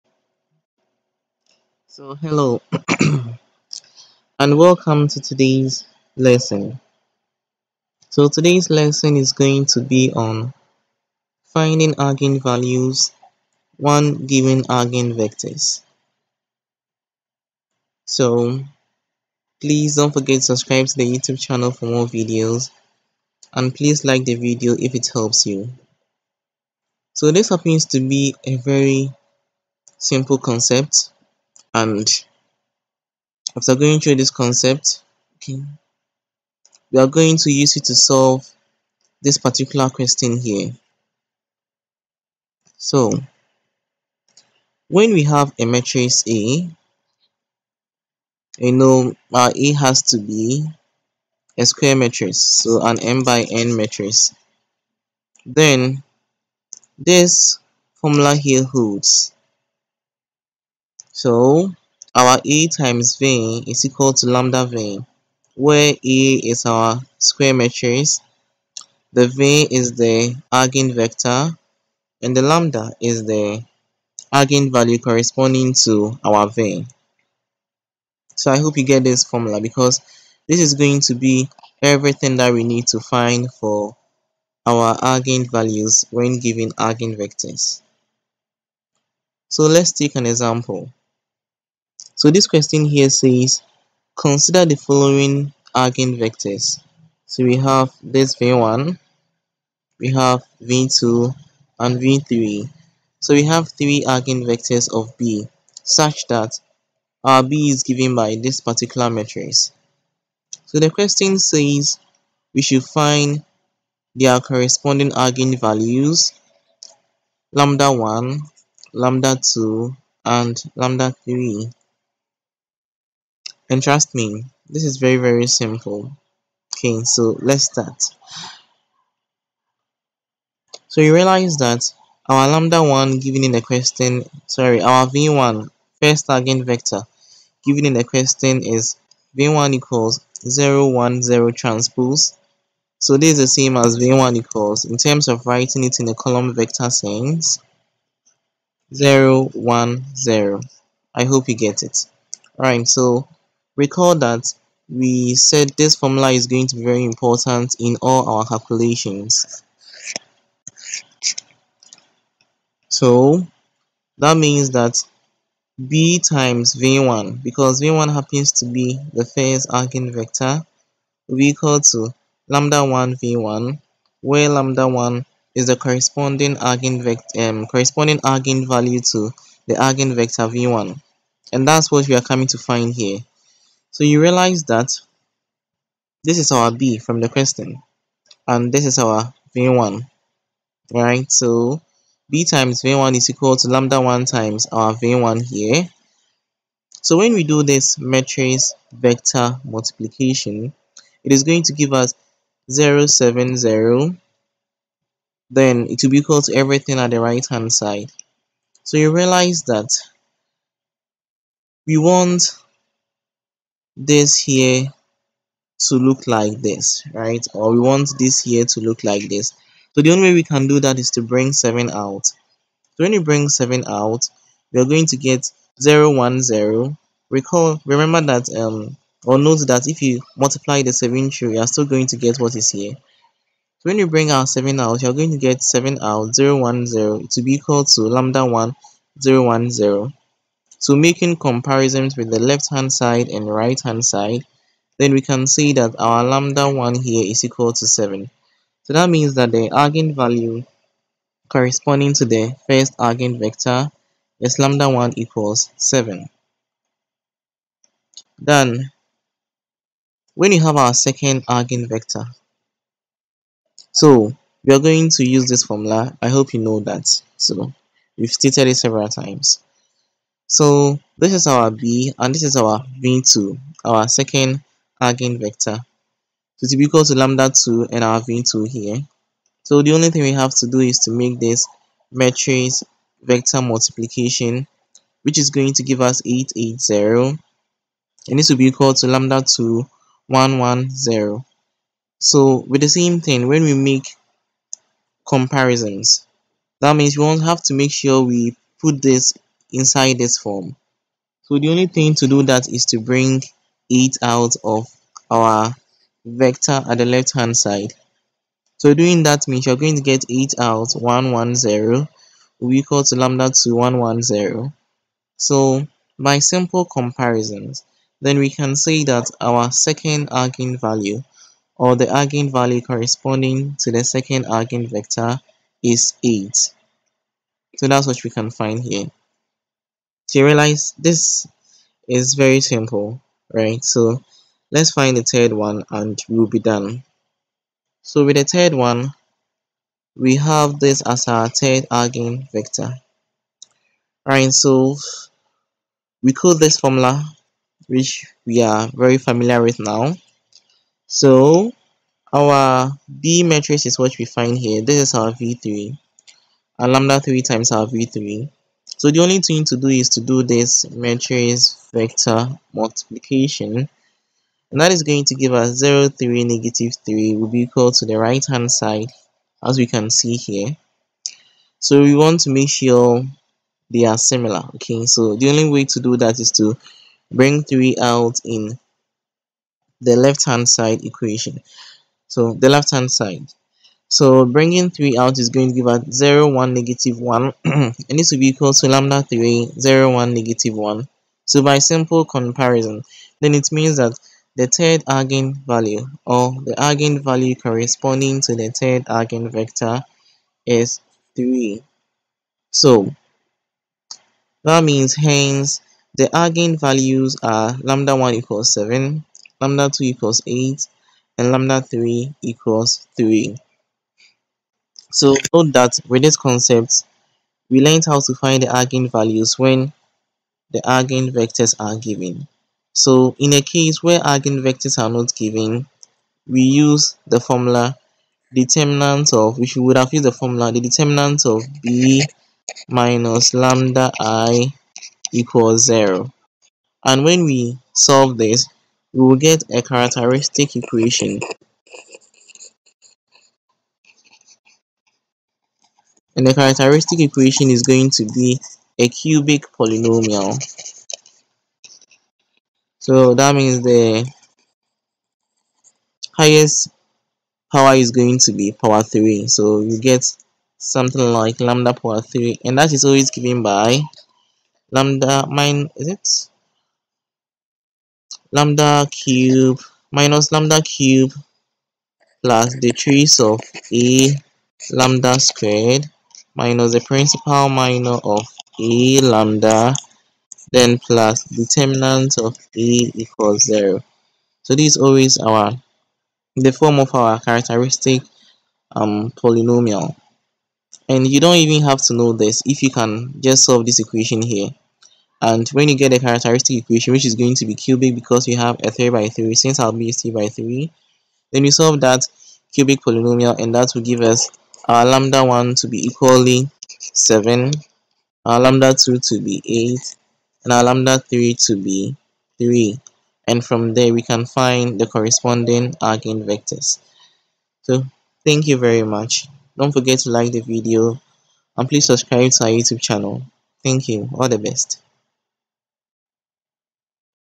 Finding the eigenvalues of a matrix given the eigenvectors
This video explains to students how to find the eigenvalues of matrix when given the corresponding eigenvectors.
Contains several examples to help you get a better understanding